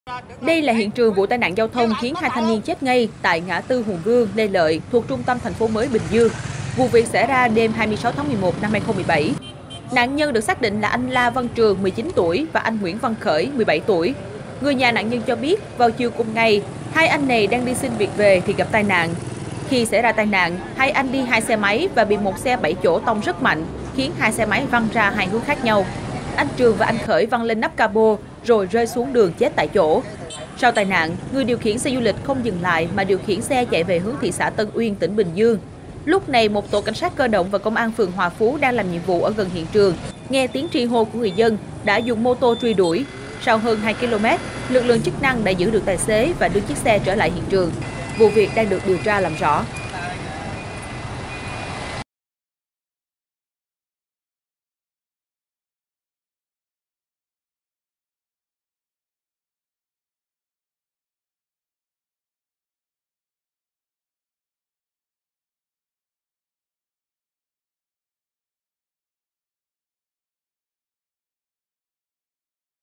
Cảnh sát truy đuổi ô tô tông chết 2 người rồi bỏ chạy ở Bình Dương
Đây là hiện trường vụ tai nạn giao thông khiến 2 thanh niên chết ngay tại ngã tư Hùng Vương - Lê Lợi thuộc trung tâm thành phố mới Bình Dương. Vụ việc xảy ra đêm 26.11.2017.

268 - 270 Nguyễn Đình Chiểu, Phường 6, Q.3, TP HCM